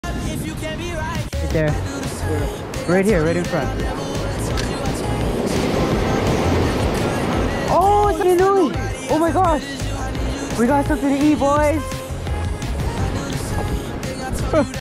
I finally caught something to eat! | HAWAII FISHING ADVENTURES
I finally caught an edible fish! This is an Nenue, also known as a Rudder-fish 🐠. You can either make raw fish or “sashimi” out of this, or you can simply fry it! Fun fact: Fishermen also use this fish as bait to catch Ulua ( Giant Trevally) or other big fish! Thank you all so much for the support, see y’all in the next video 🎣🤙🏽